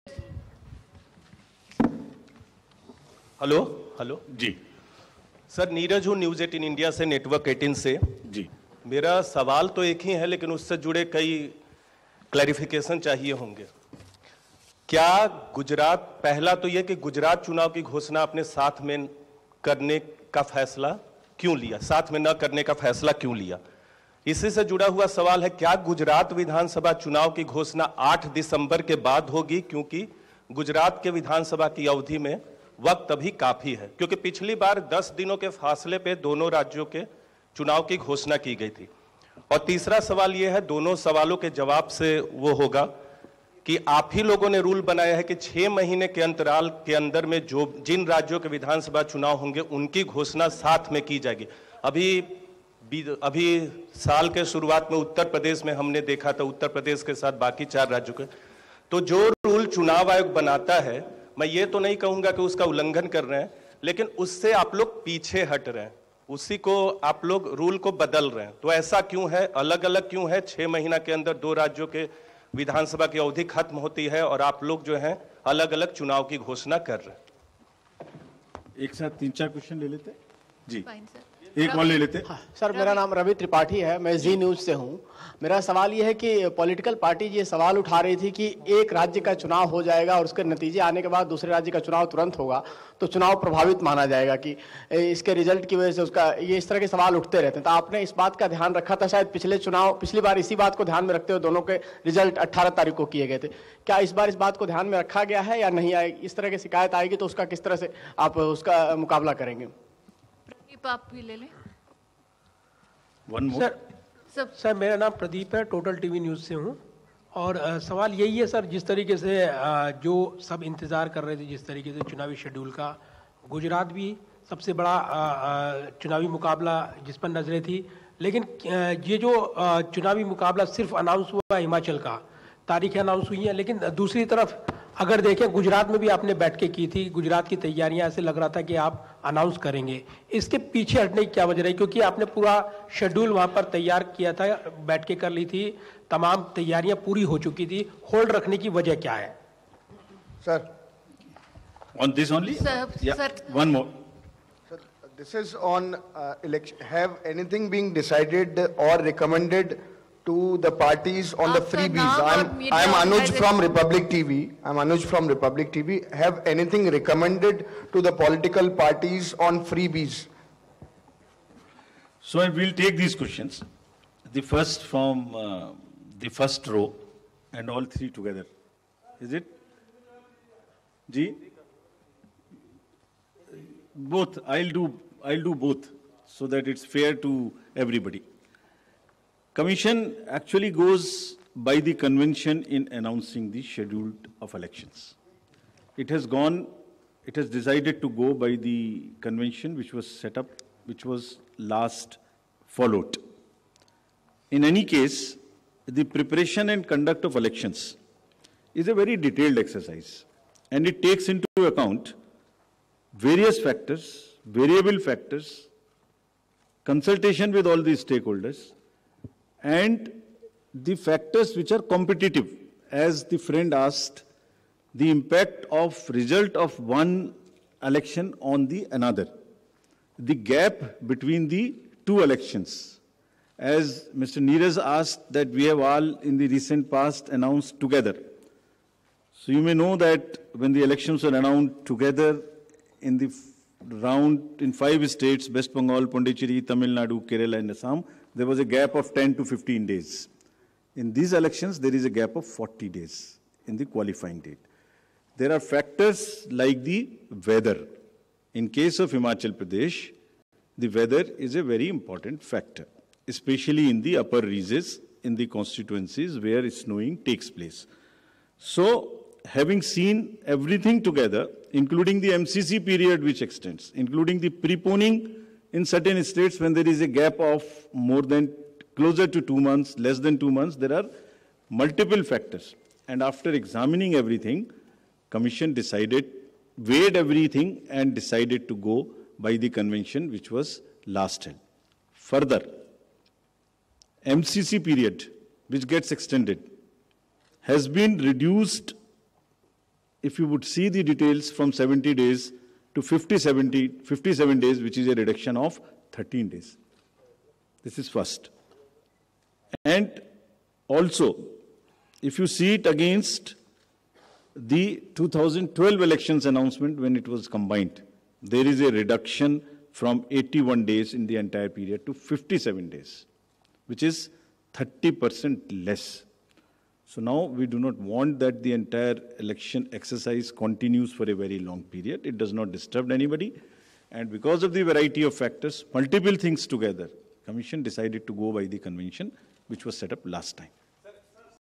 Himachal Pradesh Elections Schedule Announcement | Elections Commission Of India | English News
Himachal poll dates are announced. Polls would be in one phase and results will be announced on 8th December

Elections are to be on 12th November

n18oc_politics